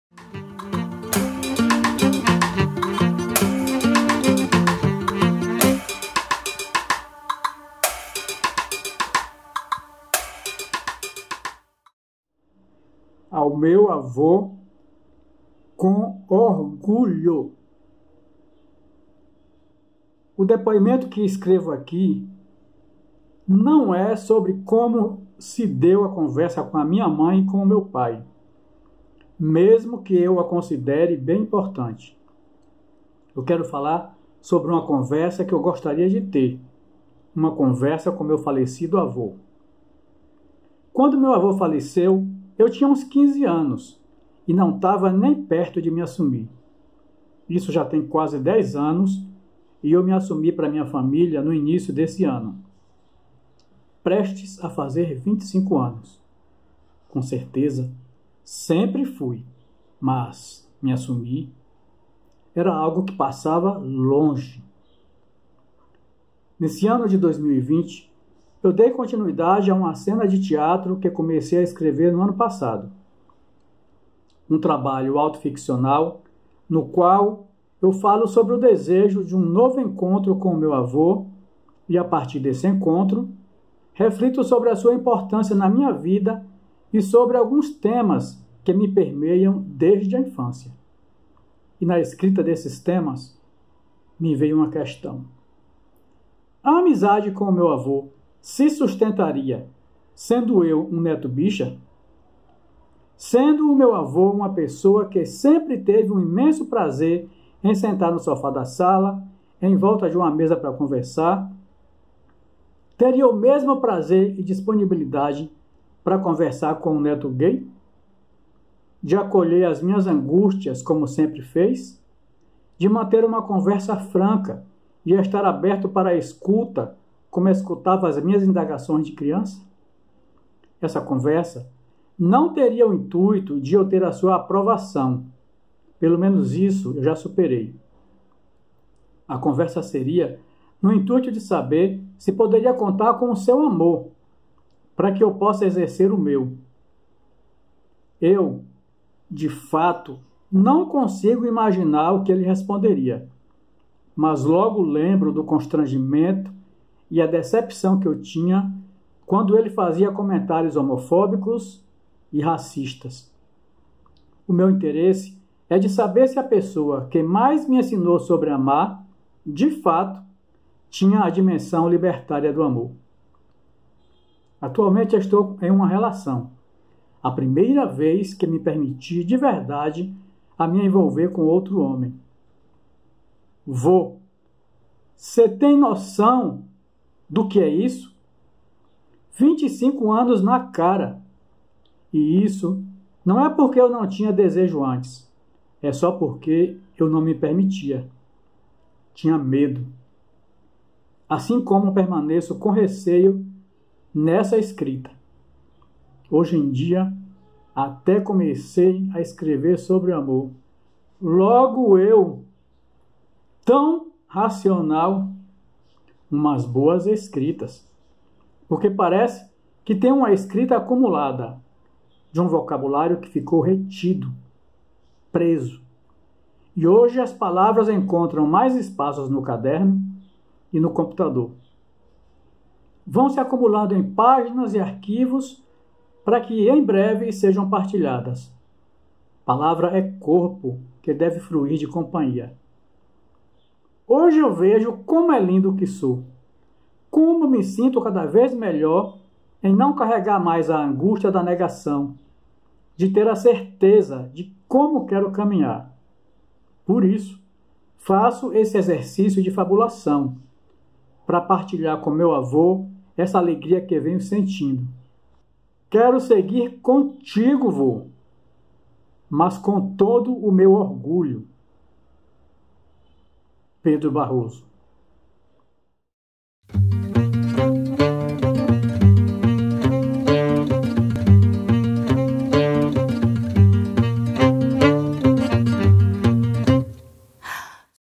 "Ao meu avô", de Valdeck Almeida
Registrar é Resistir: Crônicas do Orgulho

Série de relatos sobre assumir-se LGBTQIA+ à família. Os depoimentos dessa série foram desenvolvidos sob a orientação do escritor Marcelino Freire e o trabalho audiovisual orientado pelo Coletivo Bodoque de Cinema. A série é baseada nos relatos originais para o projeto "À minha mãe e ao meu pai, com Orgulho", do Sesc Santo André.

Episódio de hoje: "Ao meu avô", texto de Pedro Barroso e leitura de Valdeck Almeida. Outros episódios e mais sobre o projeto